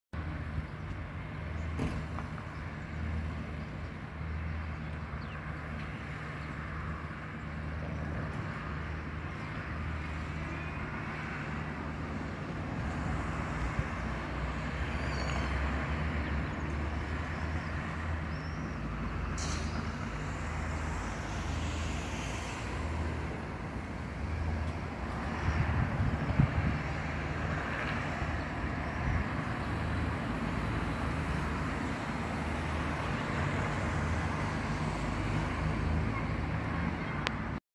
discarica in pieno centro a Pomezia 29 Maggio 2022
Piu di un mese..che siamo a Agbogbloshie, e il comune ha intenzione di portare avanti una campagna di sensibilizzazione nelle scuole, questa '' c'è più gusto a differenziare'' e quello che vedete ne è l'esempio, belle parole, la Ratta voleva riaprire la discarica di Roncigliano e ora vogliono costruire anche il ''termovalorizzatore'' a S. Palomba, non basta quello che spendiamo in tasse, fra un pò pagheremo anche in salute.